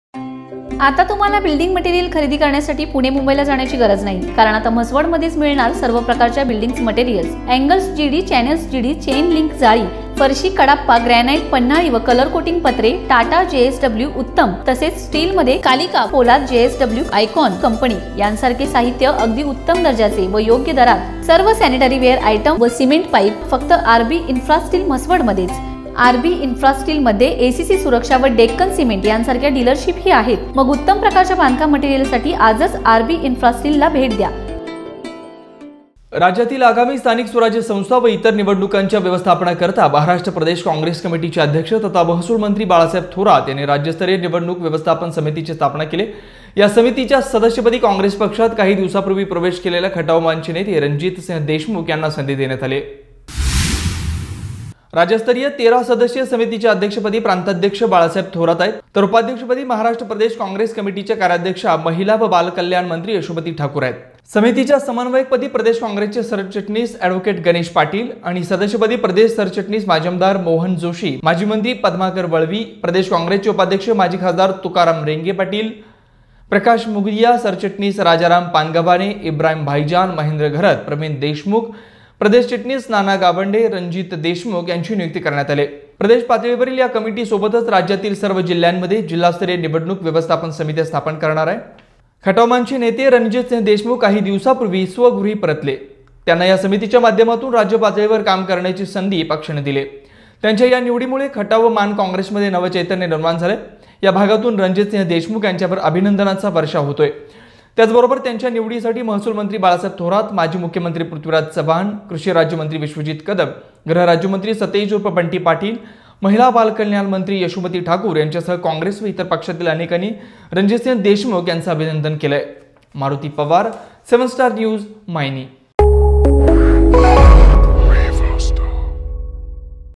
काँग्रेसच्या राज्यस्तरीय निवडणूक समितीच्या सदस्यपदी रणजितसिंह देशमुख यांची निवड
खटाव-माण कॉंग्रेसमध्ये नवं चैतन्य